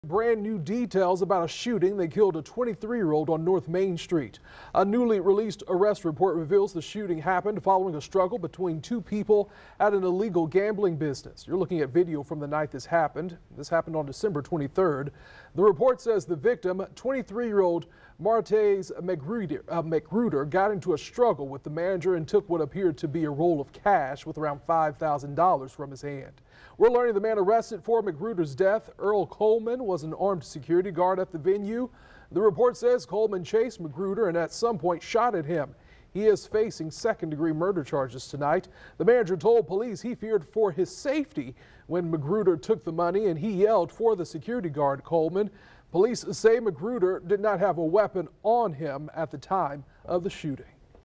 Report | Security guard of illegal gambling room in Brentwood shot, killed man after he stole $5,000
A newly-obtained arrest report shows the events leading up to a deadly shooting in December outside an apparent illegal gambling business in Brentwood.

Earl Coleman, 61, faces charges of second-degree murder and possession of a firearm by a convicted felon in connection to the shooting, according to the Jacksonville Sheriff's Office.